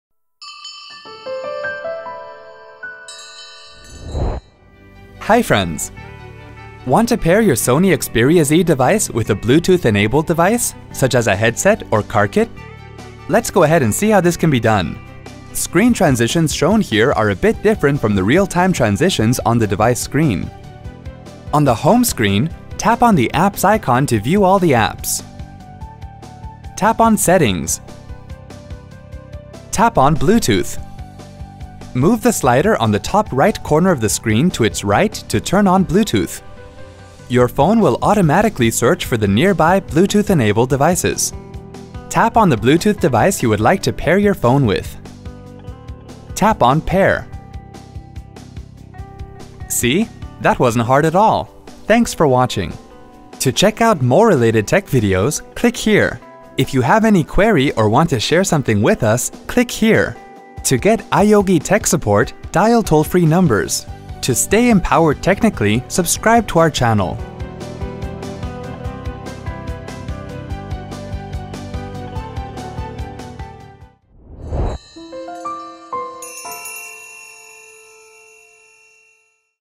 How to pair with a Bluetooth device in Sony® Xperia™ Z :Tutorial
Want to pair your Sony® Xperia™ Z device with a Bluetooth enabled device such as a headset or a car kit? Follow the steps given in this video to pair your Sony® Xperia™ Z smartphone with a Bluetooth device.